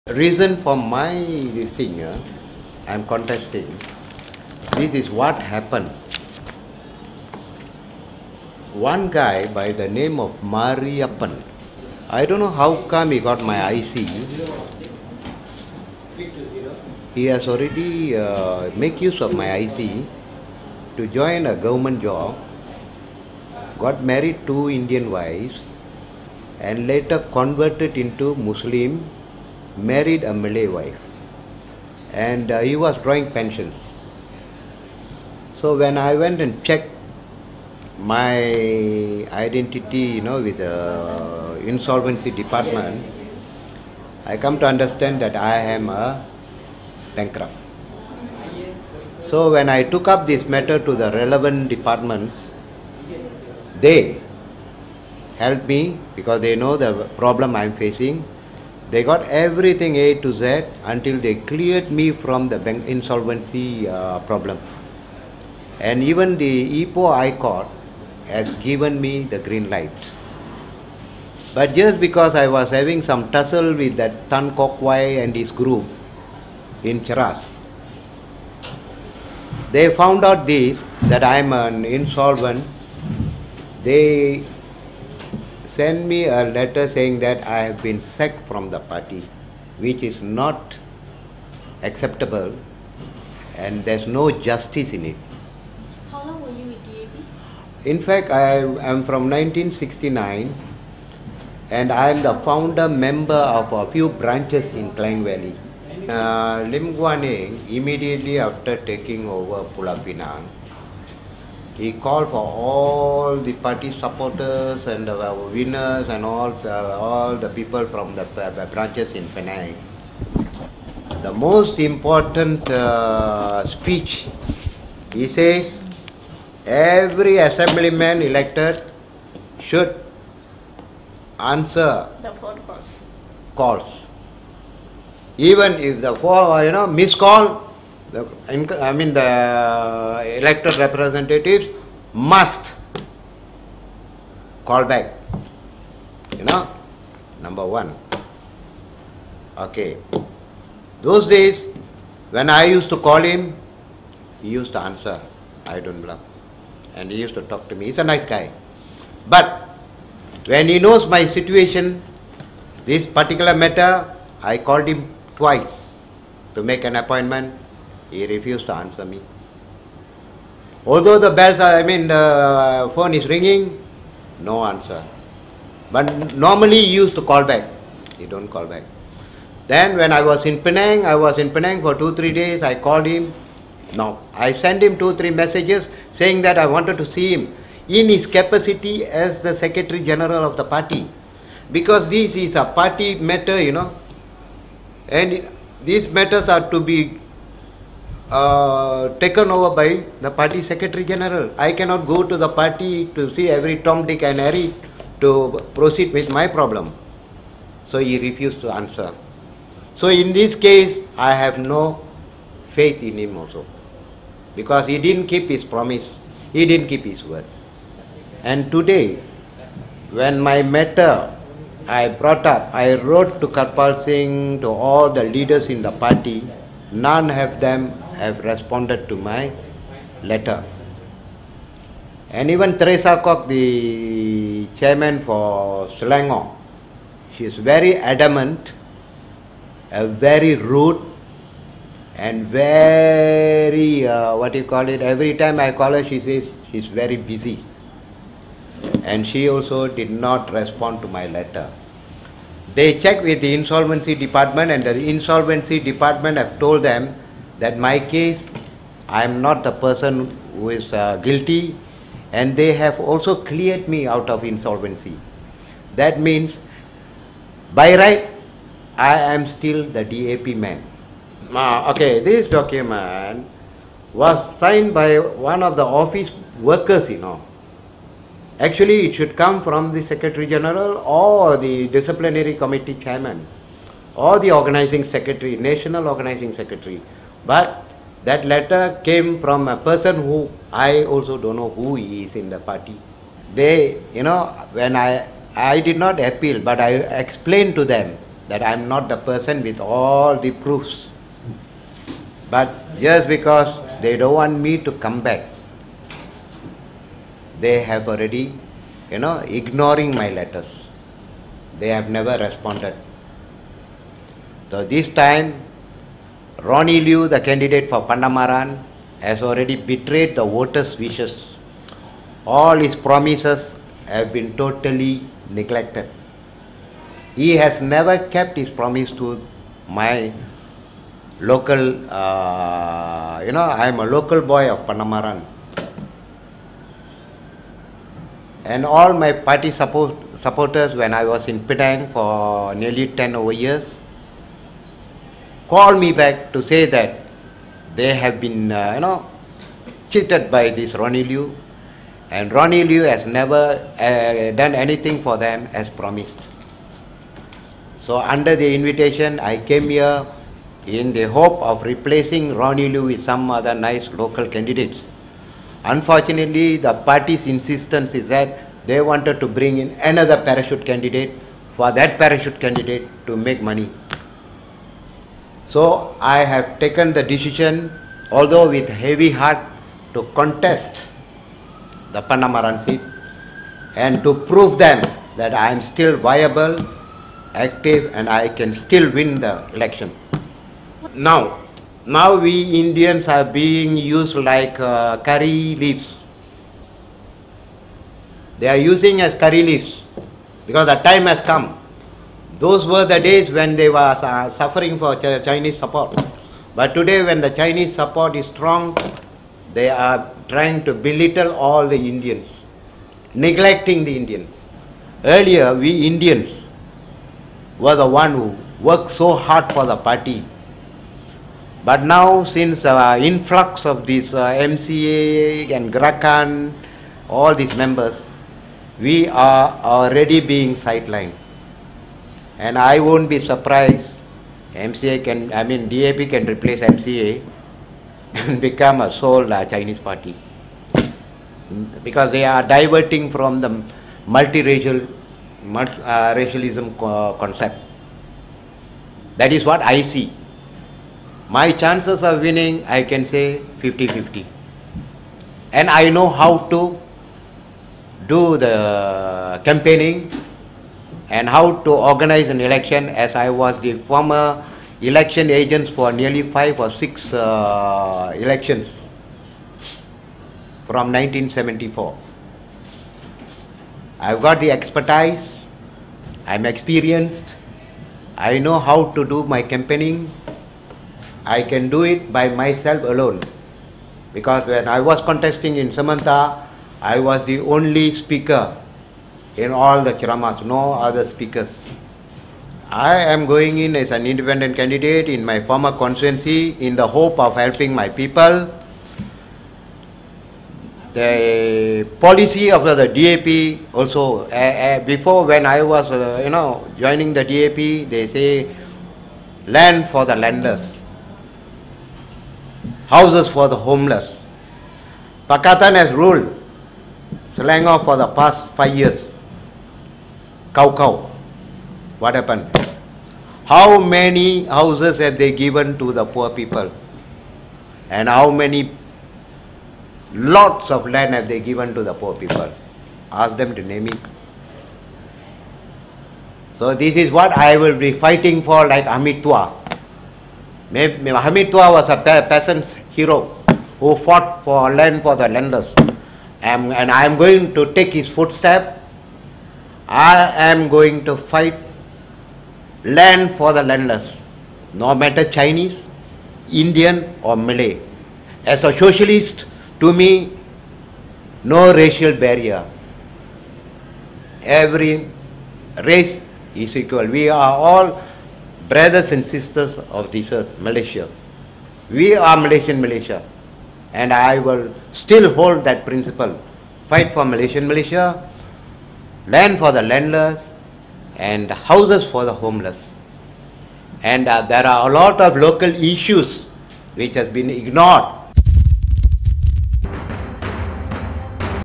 'DAP Veteran' as Independent In Pandamaran
A former DAP veteran wants to contest the Pandamaran state assembly seat in Selangor at the upcoming general election, as an independent candidate

Video by Arvind Raj